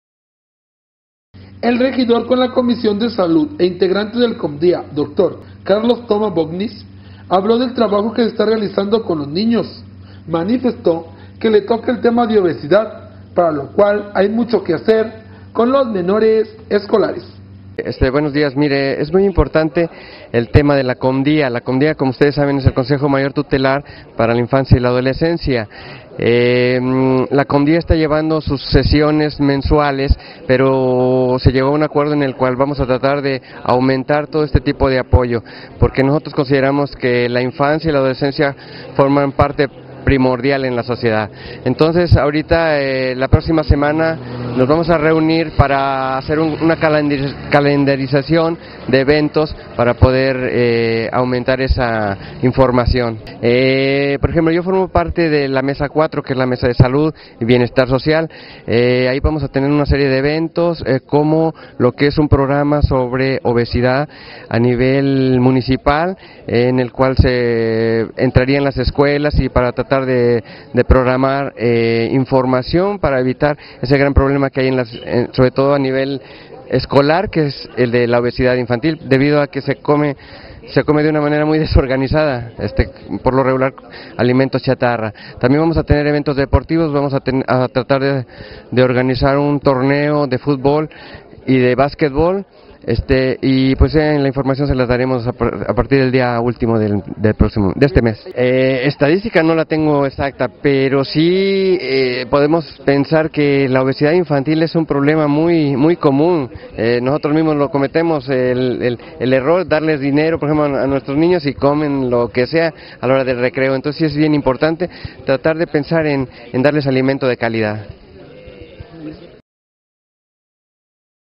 Regidor Carlos Thomas vigilante de los objetivos de la COMDIA
El regidor con la comisión de salud e integrantes del COMDIA Dr. Carlos Thomas Wognis, hablo del trabajo que se estará realizando con los niños, manifestó que le toca el tema de la obesidad, para lo cual hay mucho que hacer con los menores escolares.